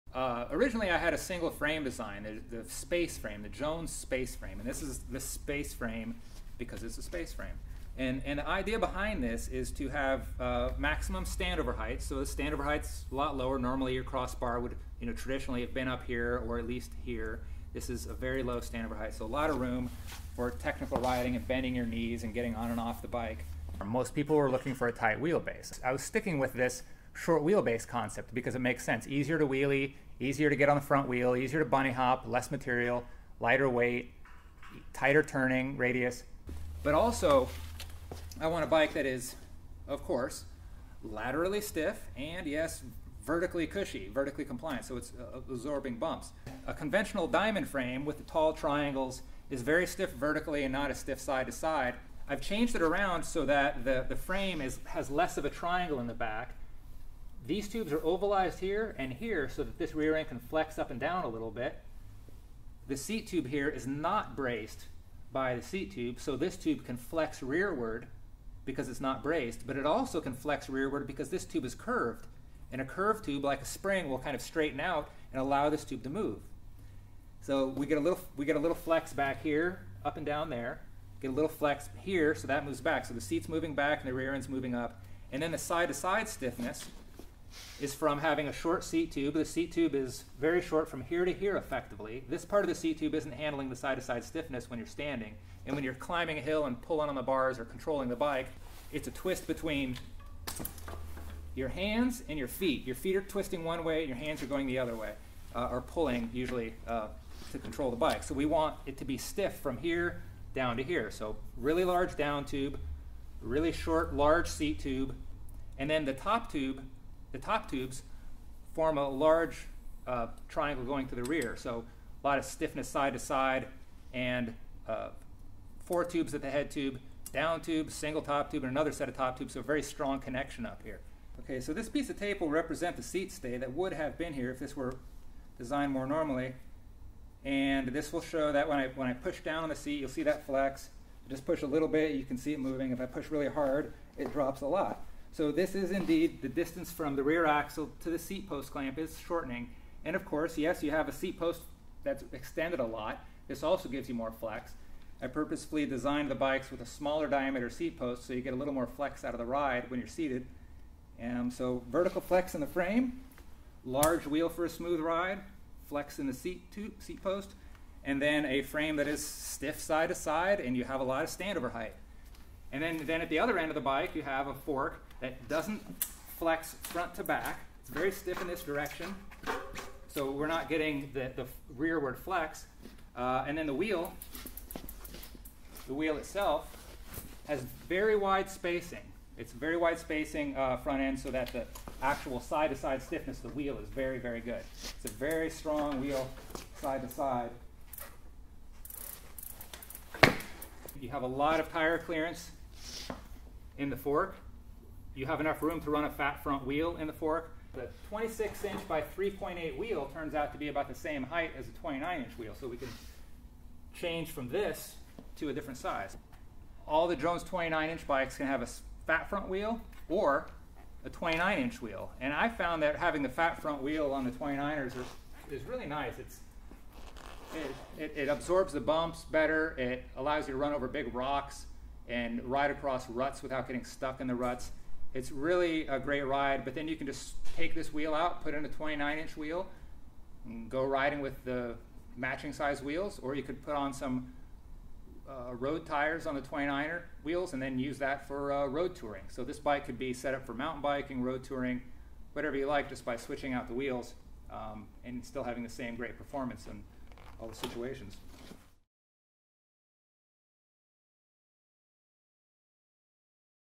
Jeff talks about the Jones 29 Spaceframe bicycle
We get a lot of questions about the Jones Spaceframe bicycle, so we compiled this video from various bits of footage we had of Jeff talking about the Spaceframe.  Jeff has considered each aspect of the design with the goal of making the ultimate short-wheelbase bike, and this video explains some of the key features that make Jones Spaceframe unique.  This isn't the whole story, but it gives a great look into the workings of this design.

Yes. It's laterally stiff and vertically compliant.

00:00 Intro
02:54 Flex, Stiffness, Strength, and Wheel Size